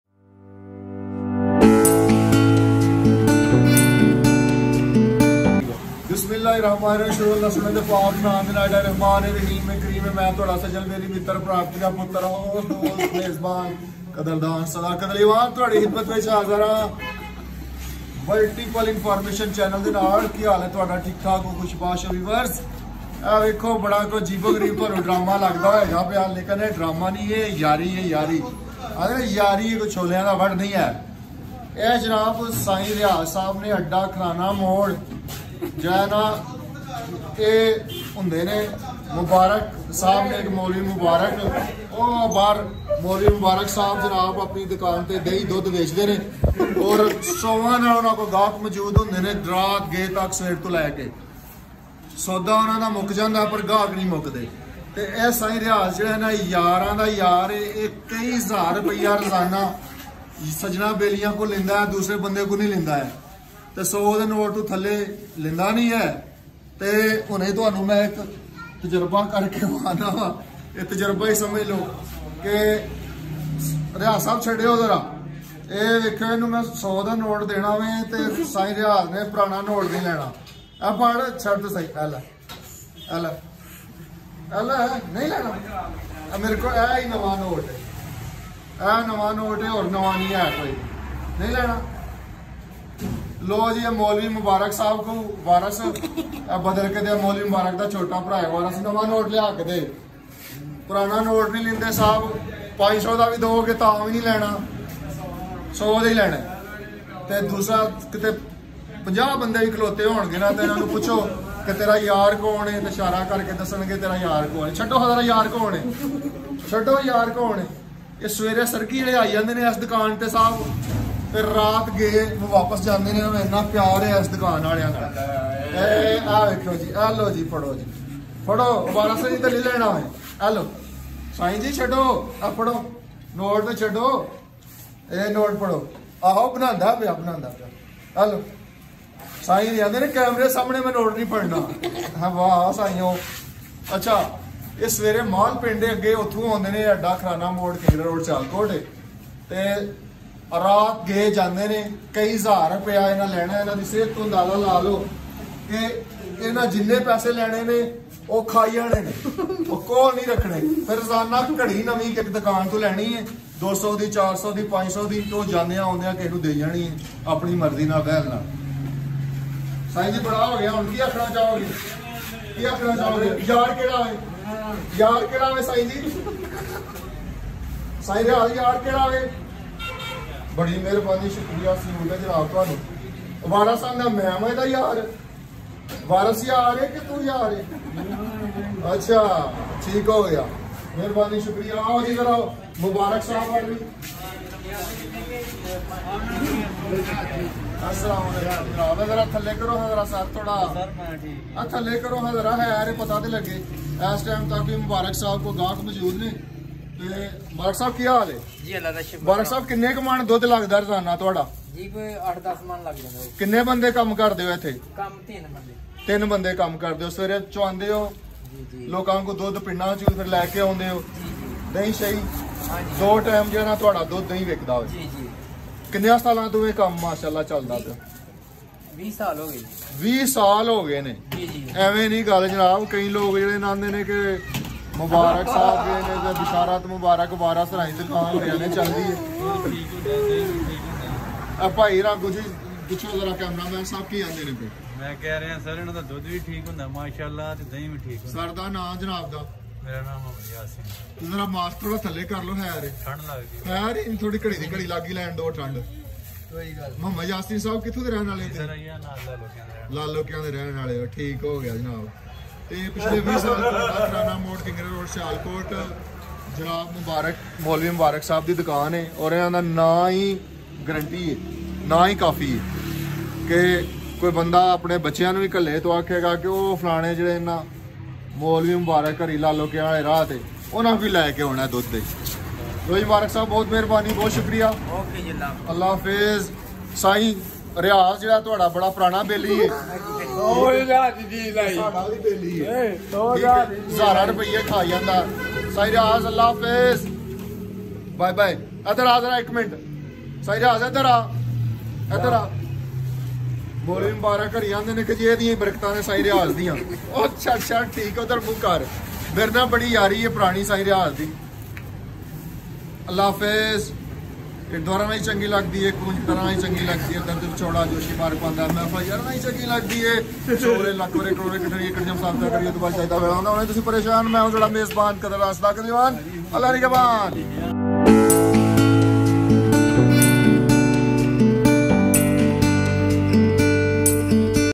سائیں ریاض کے ساتھ شغل بسم اللہ ملک شاپ اڈہ کھرانہ موڑ کنگرہ روڈ سیالکوٹ پر۔
FOR FEEDBACK ALSO COMMENT